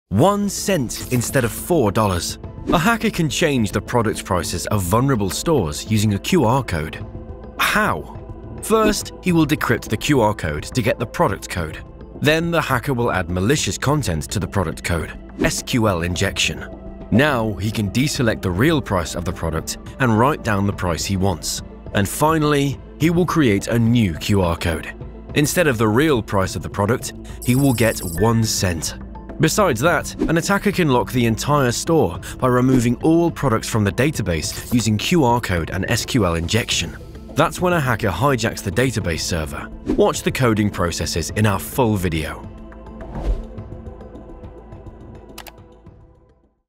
Scammer hacks the store
A hacker can change the product prices of vulnerable stores using a QR code.
How? Watch the short to know more.

Sumsub — empowering compliance and anti-fraud teams to fight money laundering, terrorist financing, and online fraud.